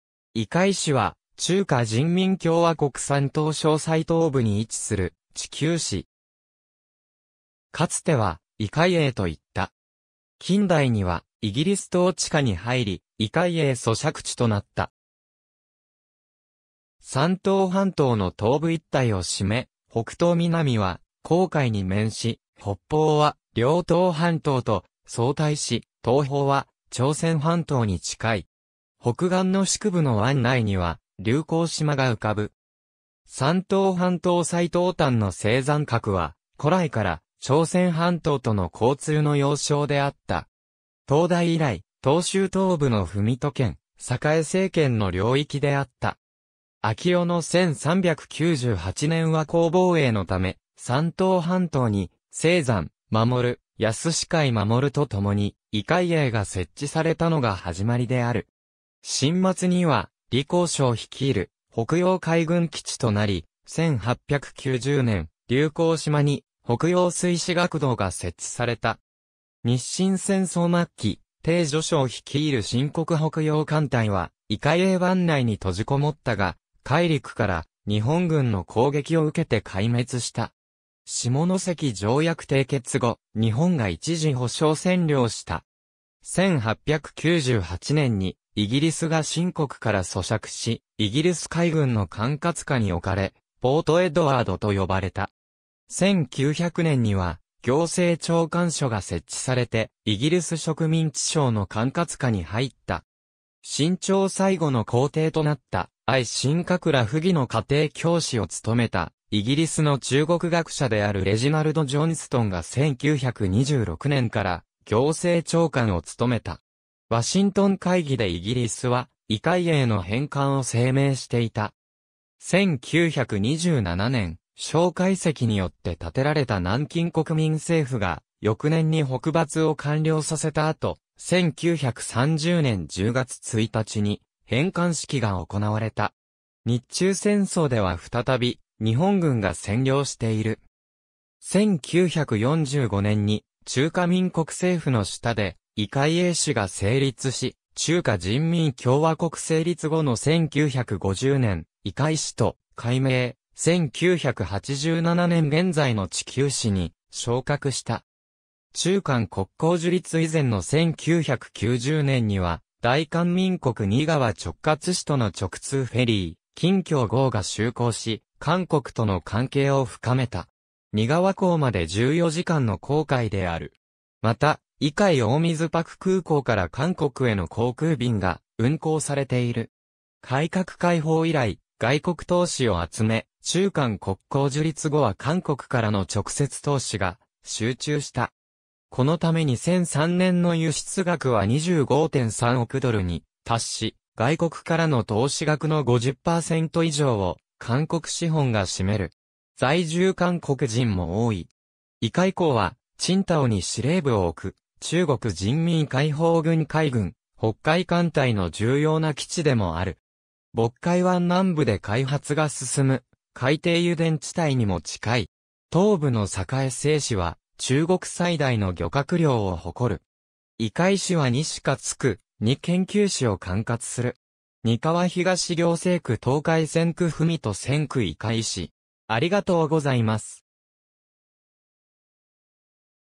威海市
威海市（いかいし）は、中華人民共和国山東省最東部に位置する地級市。
かつては威海衛（いかいえい）といった。
近代にはイギリス統治下に入り、威海衛租借地となった。
山東半島の東部一帯を占め、北東南は黄海に面し、北方は遼東半島と相対し、東方は朝鮮半島に近い。
北岸の市区部の湾内には劉公島が浮かぶ。
山東半島最東端の成山角（岬）は古来から朝鮮半島との交通の要衝であった。
唐代以来、登州東部の文登県、栄成県の領域であった。
明代の1398年倭寇防衛のため、山東半島に成山衛、靖海衛とともに威海衛が設置されたのが始まりである。
清末には李鴻章率いる北洋海軍基地となり、1890年劉公島に北洋水師学堂が設置された。
日清戦争末期、丁汝昌率いる清国北洋艦隊は威海衛湾内に閉じこもったが、海陸から日本軍の攻撃（威海衛の戦い）を受けて壊滅した。
下関条約締結後、日本が一時保障占領した。
1898年にイギリスが清国から租借し、イギリス海軍の管轄下に置かれ「ポート・エドワード」と呼ばれた。
1900年には行政長官署が設置されてイギリス植民地省の管轄下に入った。
清朝最後の皇帝となった愛新覚羅溥儀の家庭教師をつとめたイギリスの中国学者であるレジナルド・ジョンストンが1926年から行政長官をつとめた。
ワシントン会議でイギリスは威海衛の返還を声明していた。
1927年、蔣介石によって建てられた南京国民政府が、翌年に北伐を完了させた後、1930年10月1日に返還式が行われた。
日中戦争では再び日本軍が占領している。
1945年に中華民国政府の下で威海衛市が成立し、中華人民共和国成立後の1950年、威海市（県級市）と改名、1987年現在の地級市に昇格した。
中韓国交樹立（1992年）以前の1990年には大韓民国仁川直轄市との直通フェリー「金橋」号が就航し、韓国との関係を深めた。
仁川港まで14時間の航海である。
また、威海大水泊空港から韓国への航空便が運航されている。
改革開放以来、外国投資を集め、中韓国交樹立後は韓国からの直接投資が集中した。
このため2003年の輸出額は25.3億ドルに達し、外国からの投資額の50%以上を韓国資本が占める。
在住韓国人も多い。
威海港は青島に司令部を置く中国人民解放軍海軍・北海艦隊の重要な基地でもある。
渤海湾南部で開発が進む海底油田地帯にも近い。
東部の栄成市は中国最大の漁獲量を誇る。
威海市は2市轄区・2県級市を管轄する。
膠東行政区東海専区 文登専区 威海市